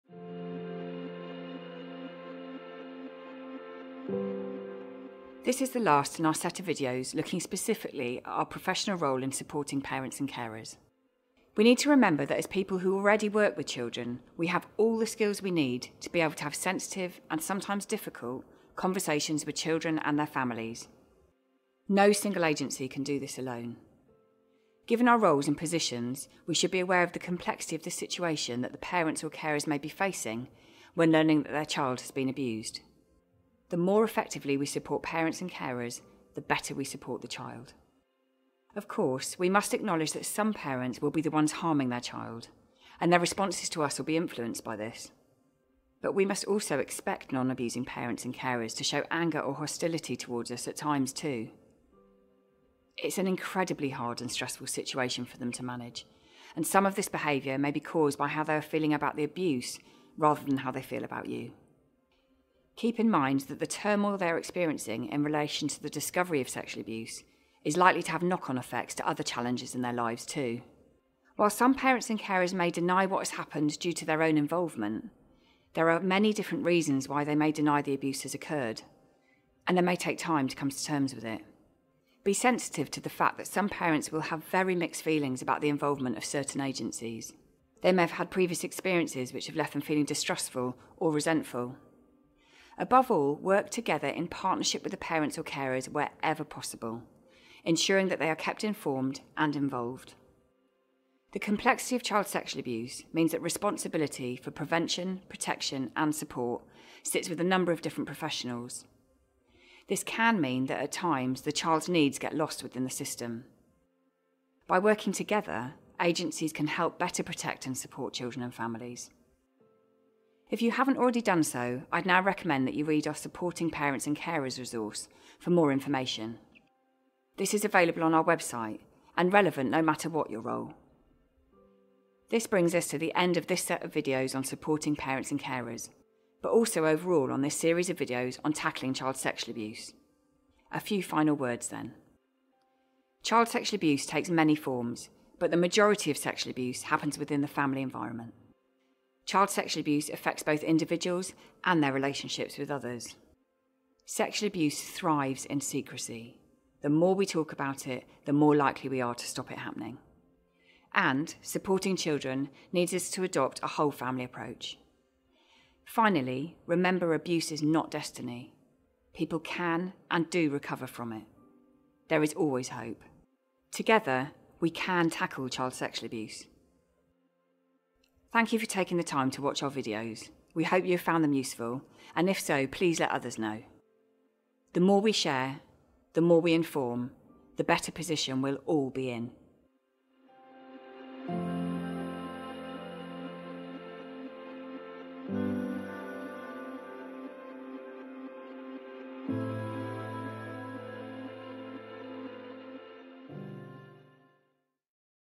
The importance of your role (12/12)
This is the final video in the Centre of expertise on child sexual abuse (CSA Centre)'s new video series which distils key information from their recent resources quickly and accessibly for professionals in 12 short videos. You can access these free professional resources